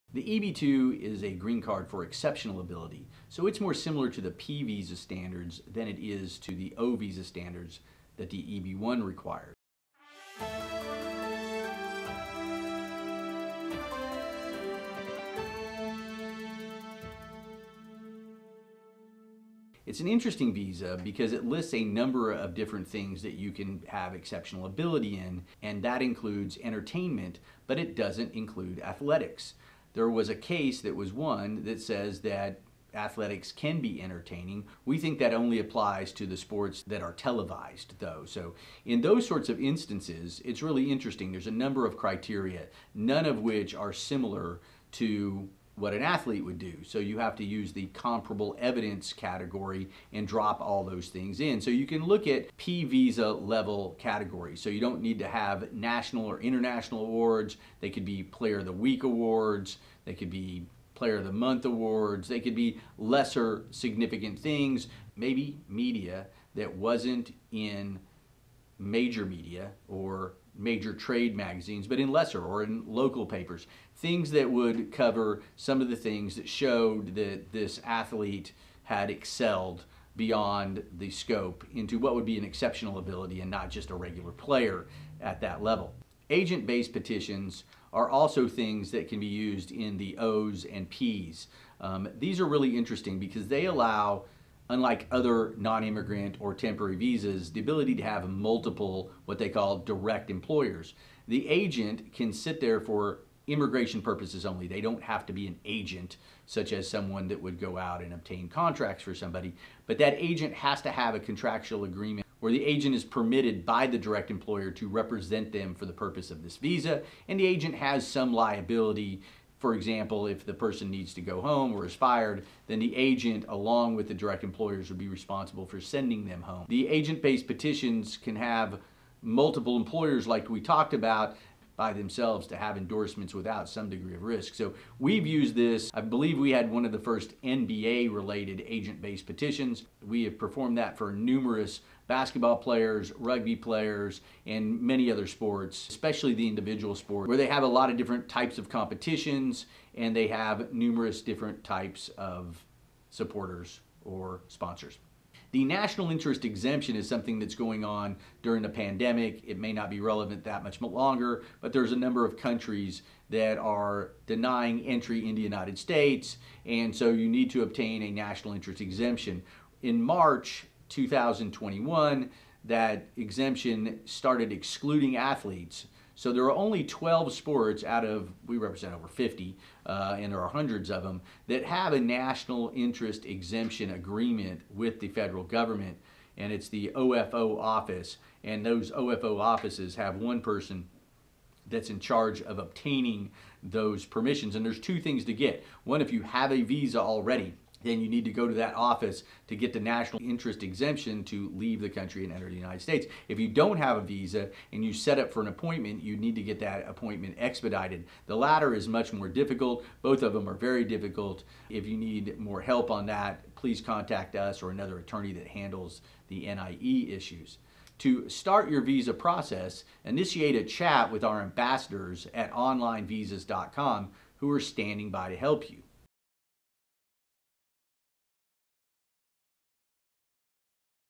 What is an EB-2 visa?
The EB-2 Visa (or EB-2 Green Card) is for Employment-Based “Exceptional Ability" or Advanced Degree Permanent Residence (Second Preference) and is available to two main types of applicants:

1. Members of professions who hold an advanced degree (or equivalent).
2. Those who can otherwise prove “exceptional ability” (i.e. expertise that is significantly higher than the norm) in their particular field, whether the sciences or arts, medicine, business, or athletics.